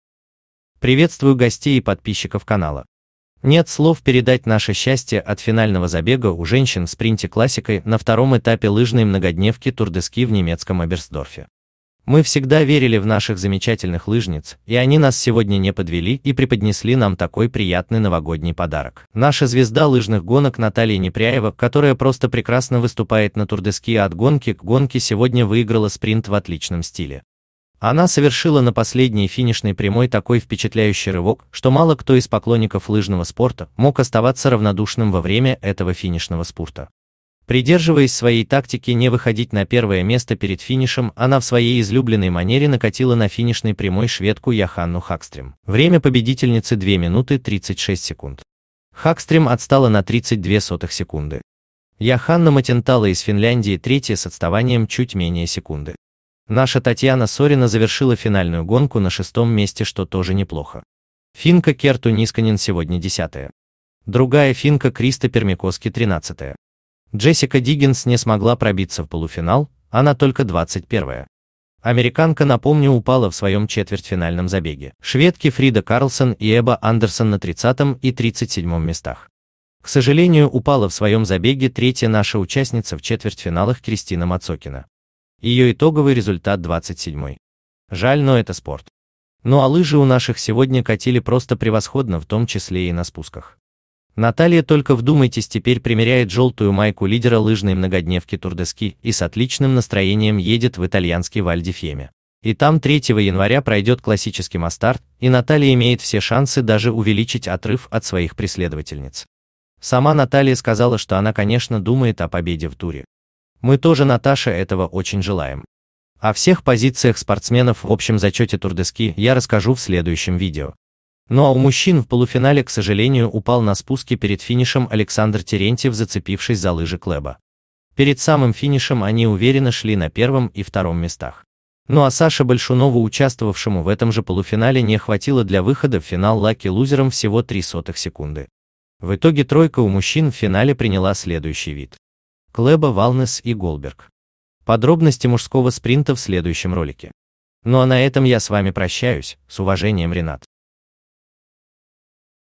Лыжные гонки. Тур де Ски. Наталья Непряева выиграла спринт в Оберстдорфе и возглавила общий зачет.
Лыжные гонки. Тур де Ски 2021-22. Наталья Непряева выиграла спринт классикой в Оберстдорфе и возглавила общий зачет Тур де ски. Следующий этап пройдет в итальянском Валь ди Фьемме. Там пройдут два масс старта.